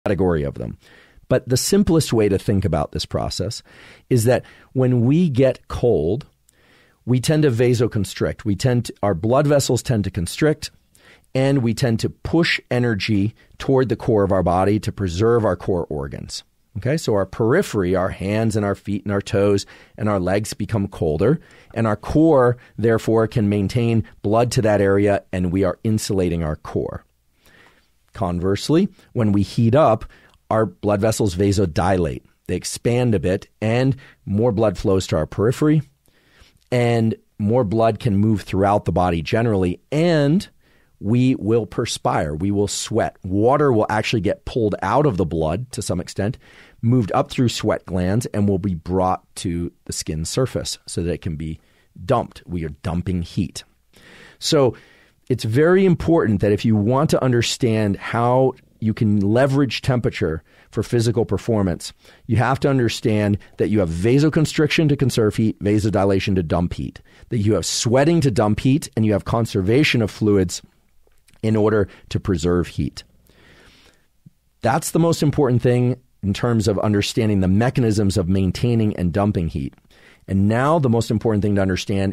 Master Vasoconstriction & Vasodilation
The speaker delves into the physiological processes of vasoconstriction and vasodilation, explaining how these mechanisms help regulate body temperature during physical exertion. Emphasizing the importance of understanding these processes, the discussion highlights how they can be leveraged to optimize physical performance and manage heat effectively.